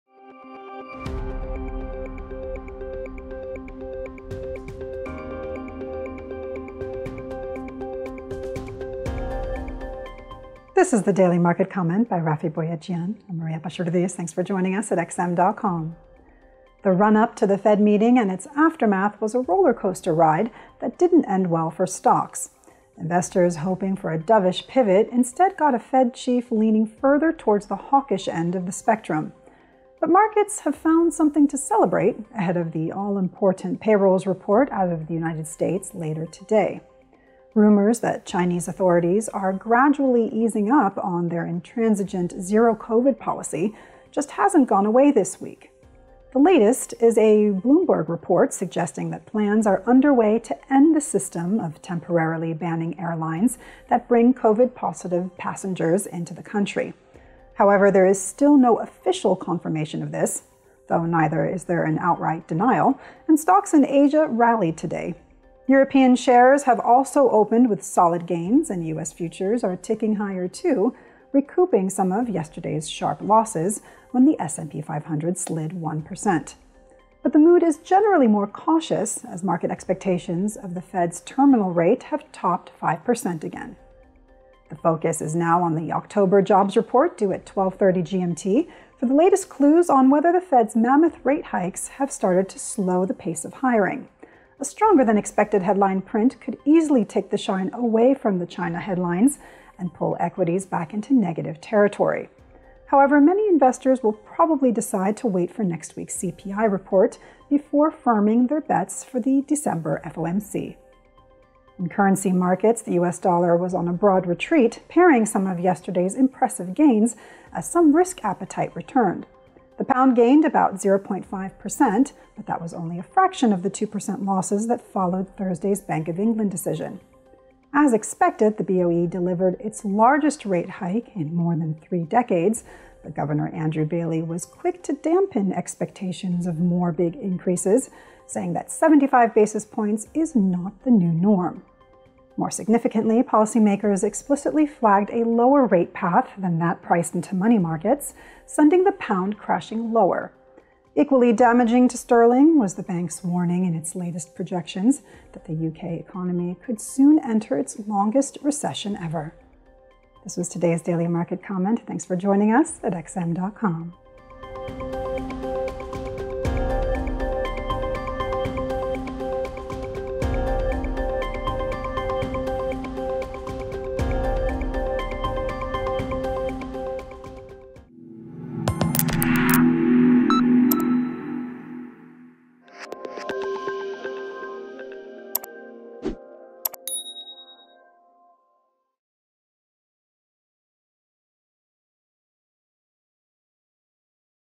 Forex News: 04/11/2022 - Stocks pare losses ahead of NFP on China hopes; BoE sinks pound
Dollar off highs, stocks turn positive as China reopening chatter lifts mood.
But NFP report could bring Fed tightening fears back into the fore. Pound
left reeling after BoE hikes big, warns of long recession.

0:00 Intro
0:16 Stocks
1:23 Nonfarm payrolls report
1:50 Pound and BoE
2:44 Events ahead

For more information on the topics discussed in this video, including detailed charts and tables, read the full Daily Market Comment article: